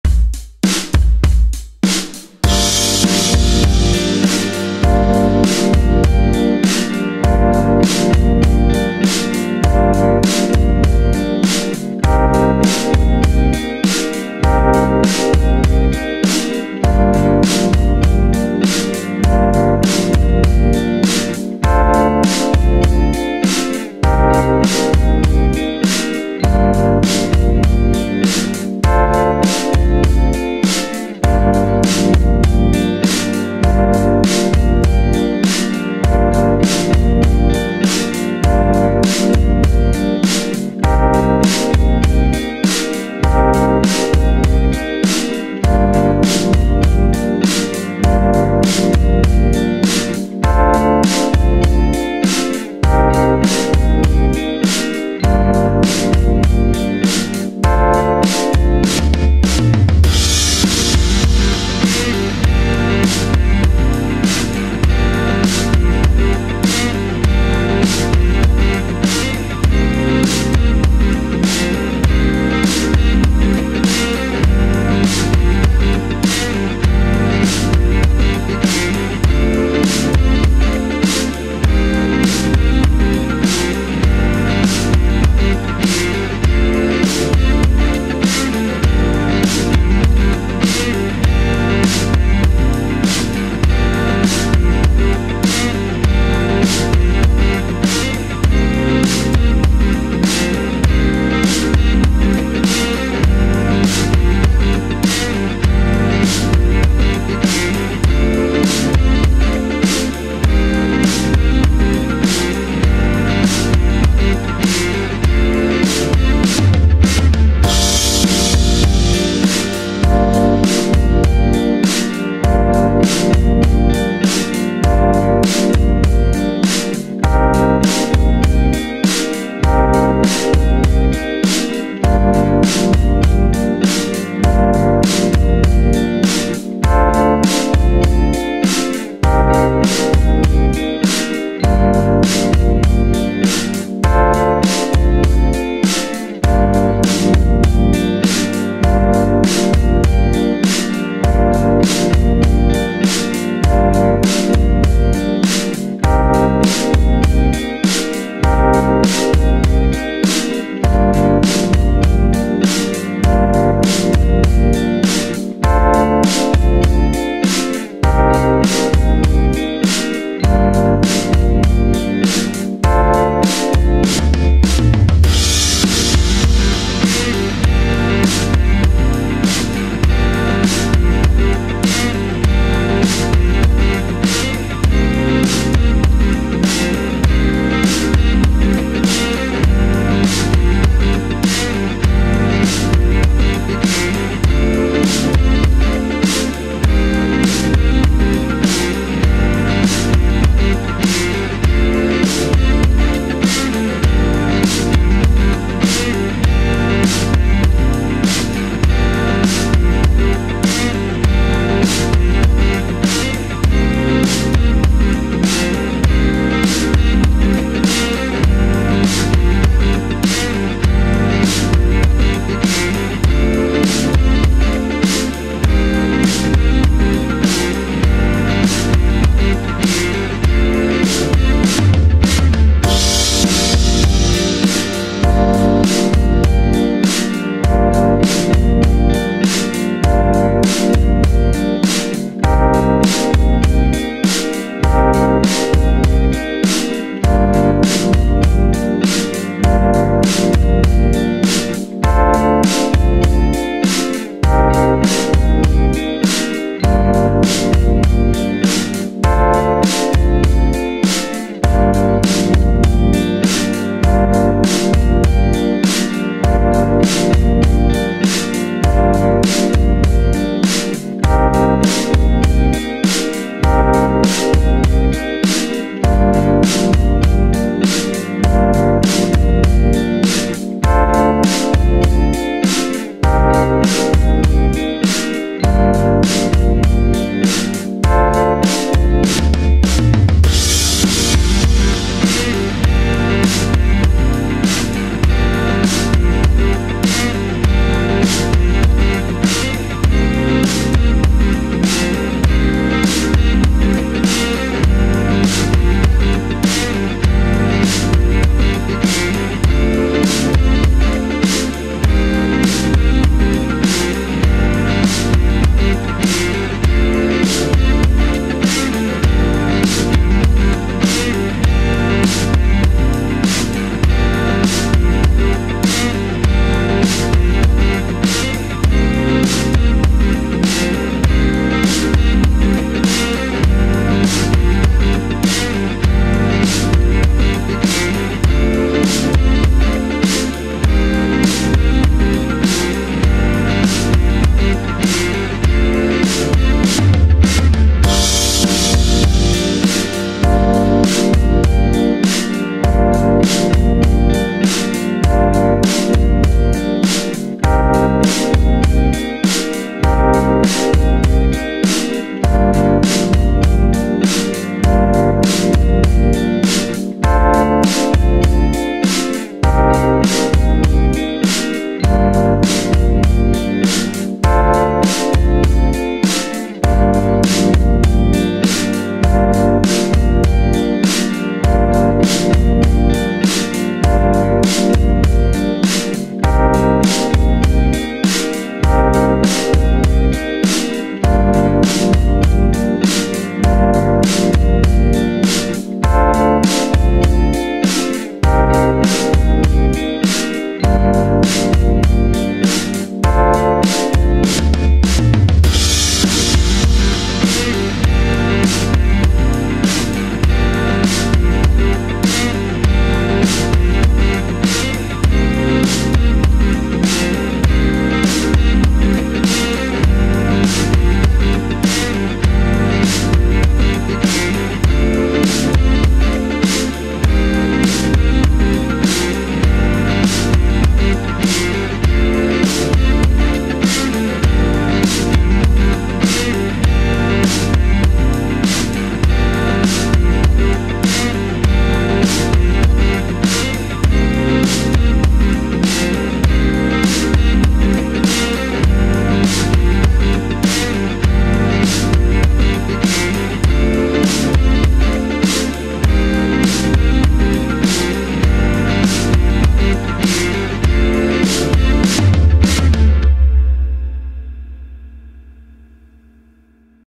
Groovy Blues Backing Track In Gm
Groovy Blues Backing Track In Gm for solo or lead practice or improvisation.
This Backing Track is made for guitar, piano, synth, vocals etc.

Image by StockSnap from Pixabay
Imagen de Alejandro Alvariño en Pixabay